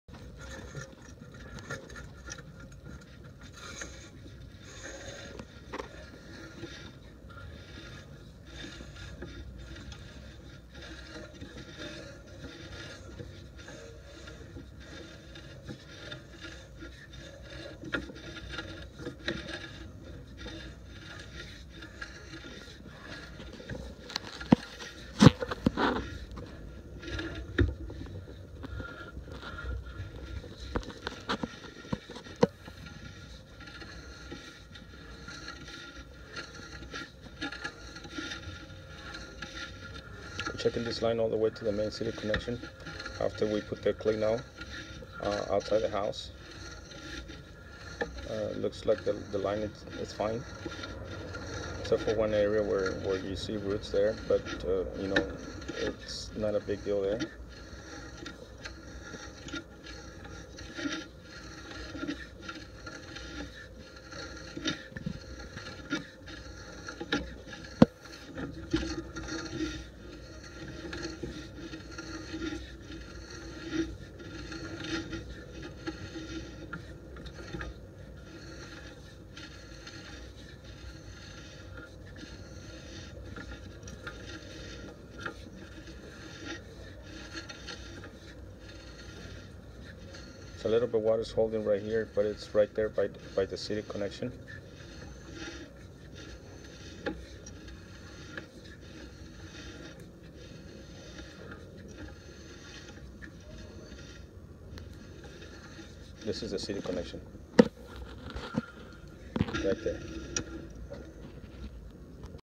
4836 w 140th st
Jet Main sewer line from new clean out